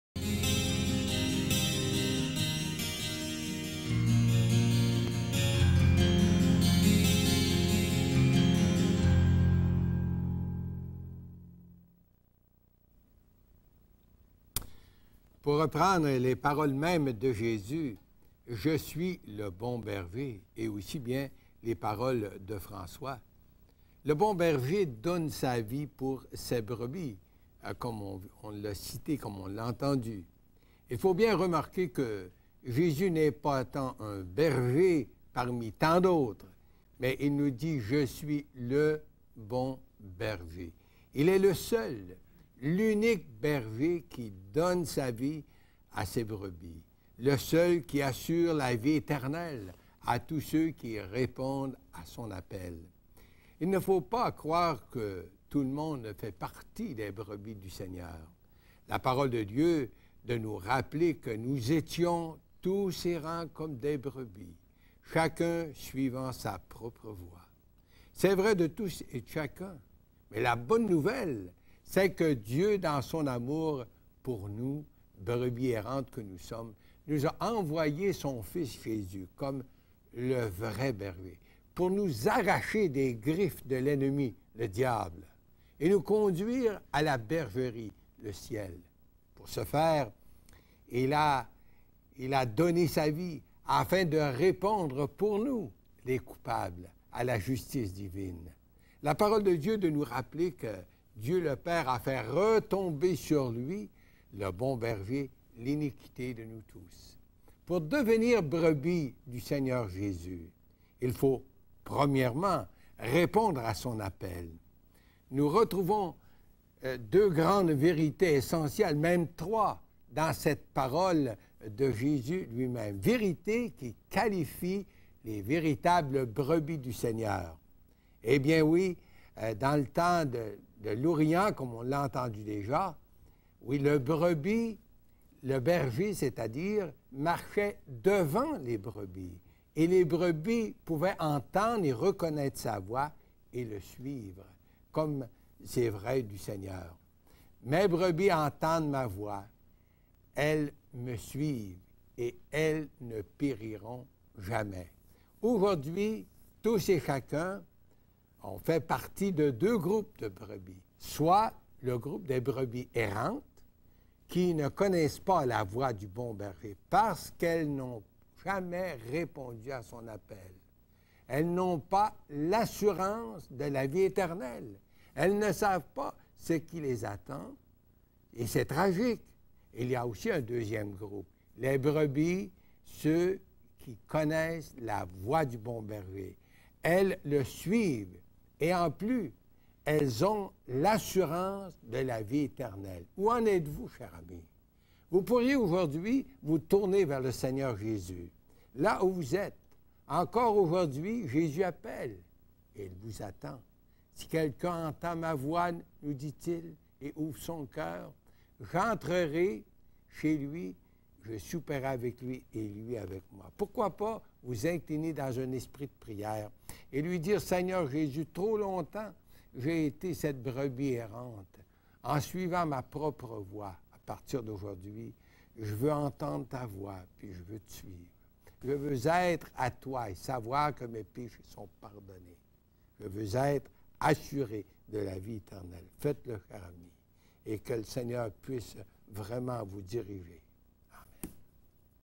Que ferez-vous du vrai Jésus ? (05)
Saison 2000
Émission No.05
Thème : Qui est le vrai Jésus ?
Message: Que ferez-vous du vrai Jésus ? (05)
Avec Gaston Jolin, Jean-Pierre Cloutier & François Fréchette